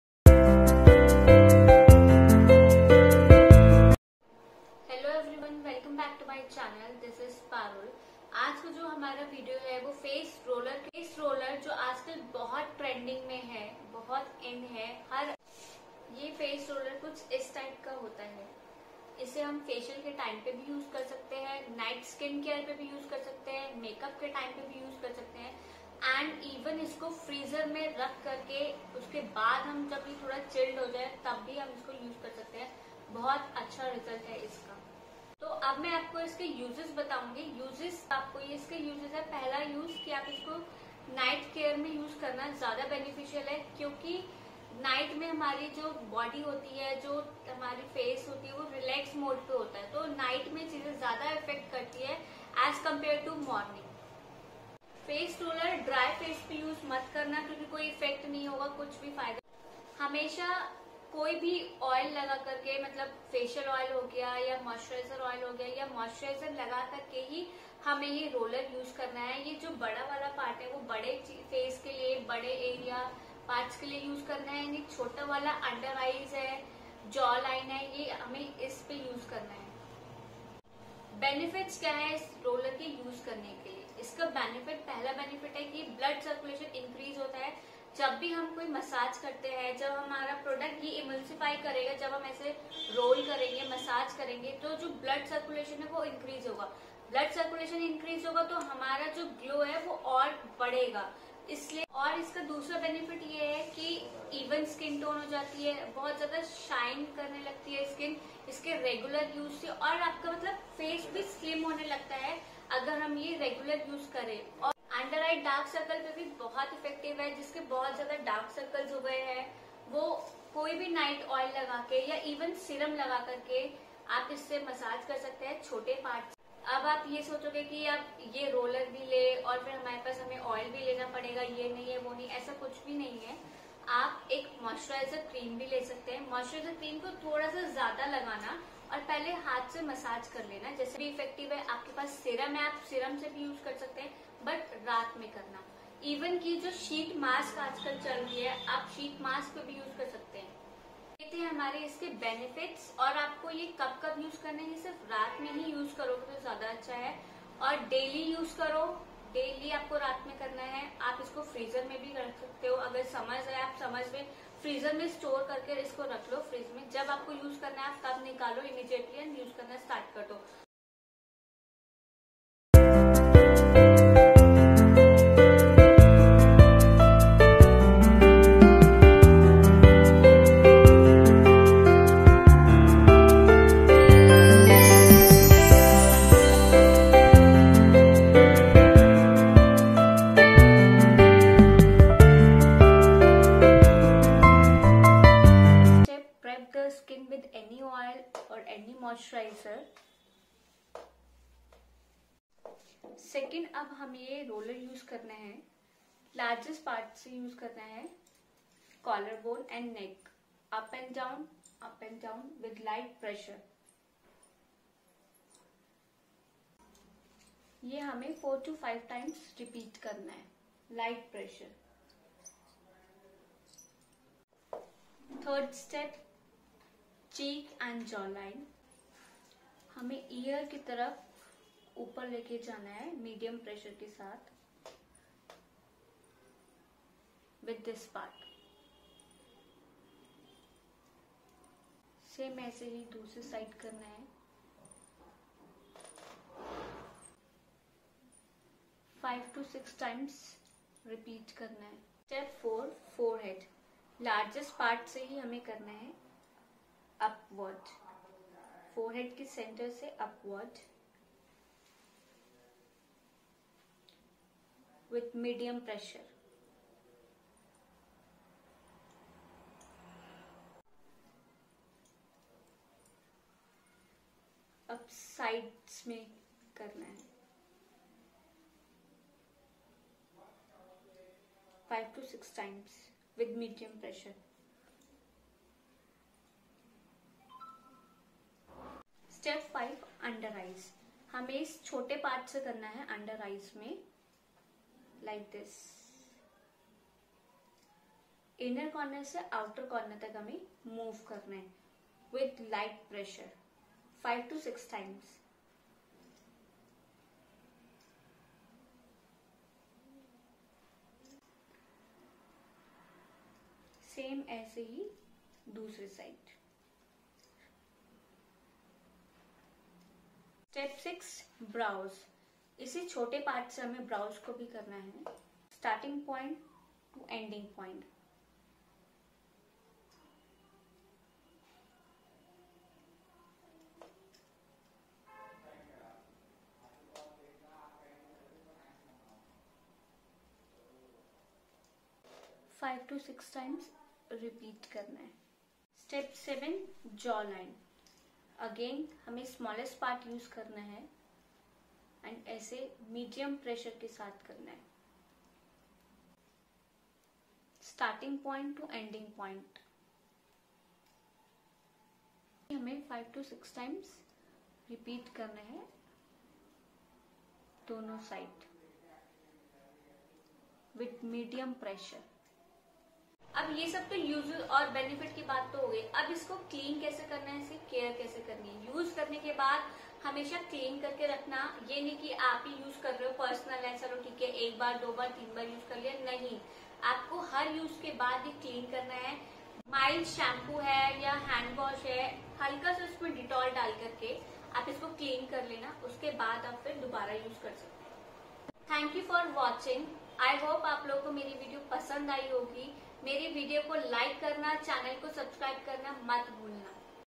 How to use Jade roller in easy steps
In this video , I am sharing how to use face roller .
You can use on your skin to help you look young by doing on a regular basis along with eating right and taking proper sleep.